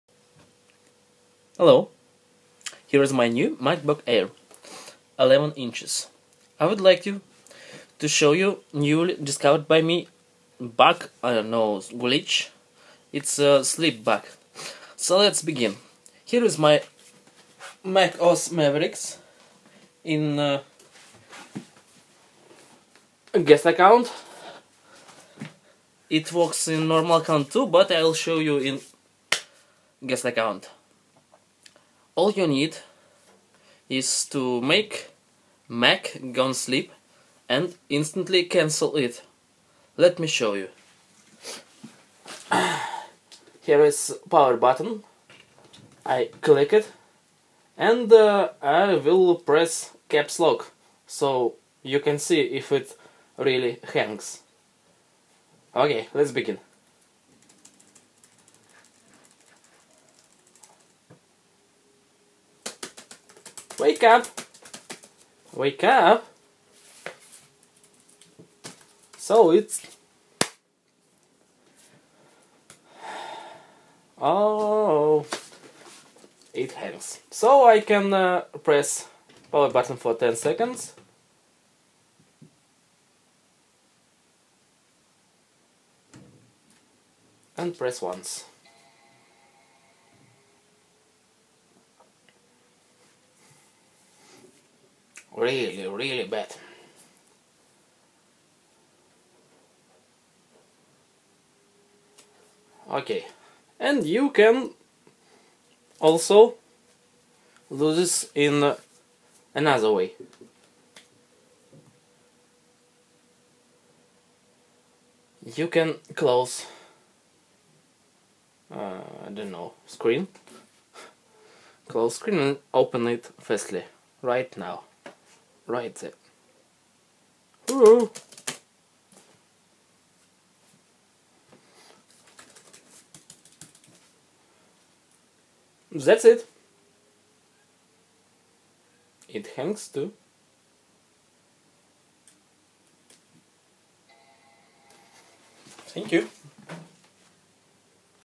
MacBook Air 2013 Mavericks sleep bug/glitch
Tested by me on 11" and 13" of middle 2013 macbook airs.
Possible will work on late 2013 macbook pros.
Also work when lock screen appears after sleep. So you can ruin someone else's work by doing this on even locked computer.